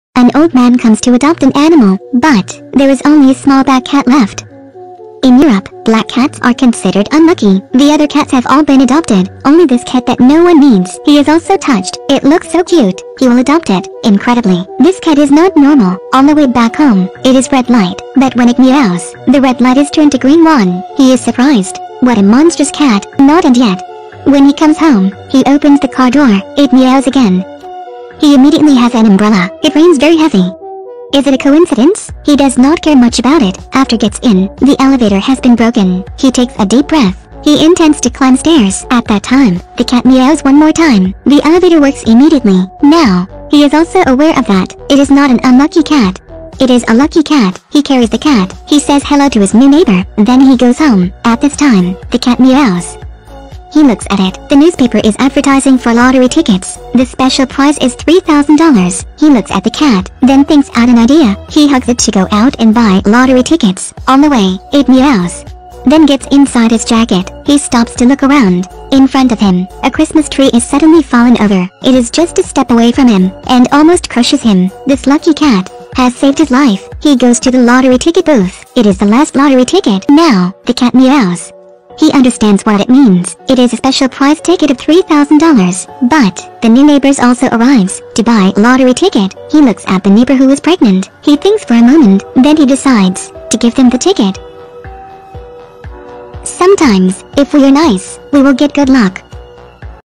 NO ONE DARES TO ADOPT A BLACK CAT FOR FEAR OF TROUBLE !!! Staatsloterij (Dutch State Lottery)
The latest New Year's Eve campaign from Staatsloterij (Dutch State Lottery). The main role in the campaign, themed "Believe in Luck", is once again given to an animal. This time it's a black cat called Frummel. The kitten remains as the last of its litter. But once he's adopted, it turns out that black cats don't always bring bad luck...